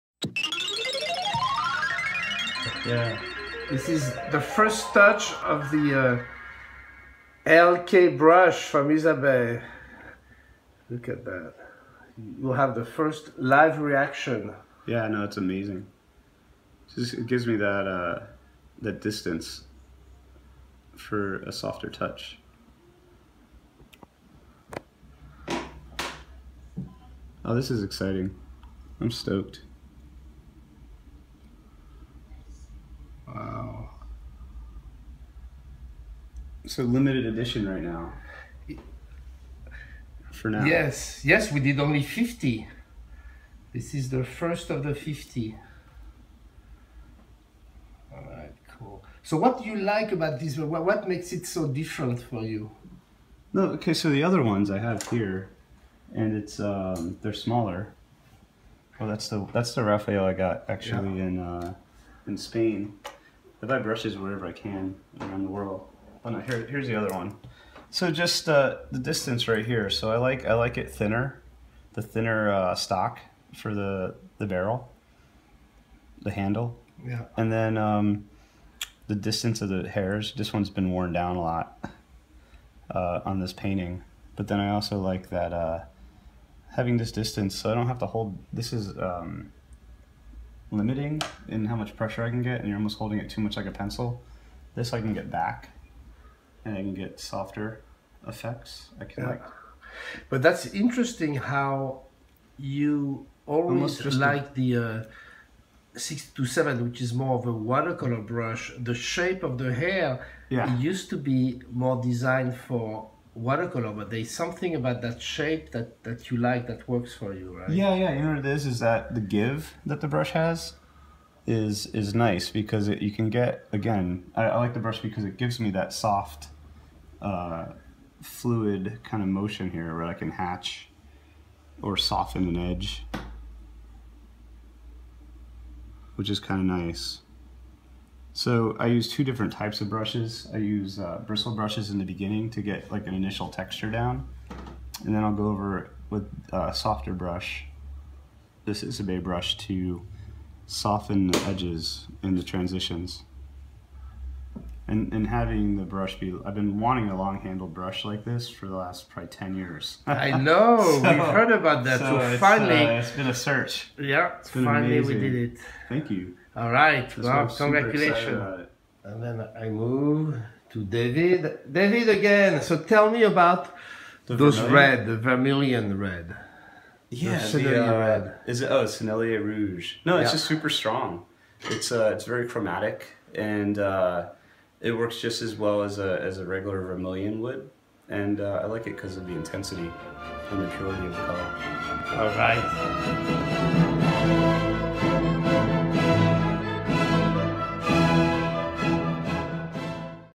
Pierre x David Jon Kassan
Follow our fearless leader as he speaks with the exceptionnal David Jon Kassan about Sennelier, and Isabey. David is discoverint for the fist time a brand new Isabey brush. Do you think he liked it?